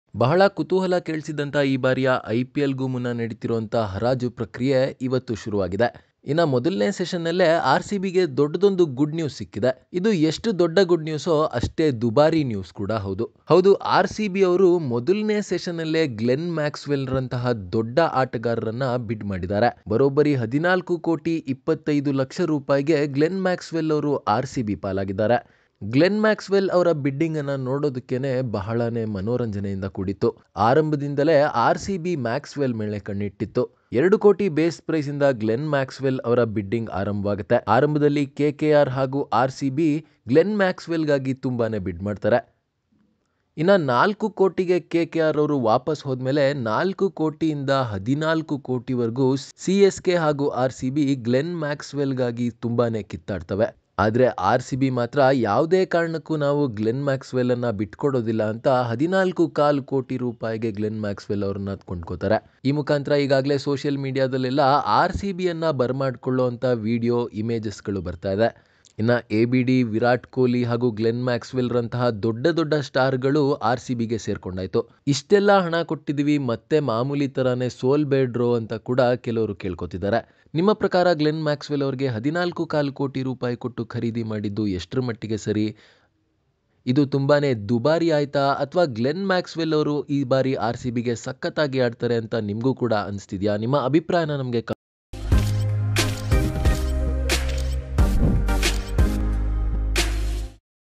Maxwell ಇನ್ಮುಂದೆ ರಾಯಲ್ ಚಾಲೆಂಜರ್ | Oneindia Kannada
ಇಂಡಿಯನ್ ಪ್ರೀಮಿಯರ್ ಲೀಗ್ (ಐಪಿಎಲ್) 2021ಗಾಗಿ ಫೆಬ್ರವರಿ 18ರಂದು ಚೆನ್ನೈನಲ್ಲಿ ಹರಾಜು ಪ್ರಕ್ರಿಯೆ ನಡೆಯುತ್ತಿದ್ದು.ಕೆಲವು ಸ್ವಾರಸ್ಯಕರ ಹರಾಜು ಸನ್ನಿವೇಶಗಳು ಇಲ್ಲಿವೆ
Ipl is just around the corner and today auction gave us a clear picture regarding all the teams this year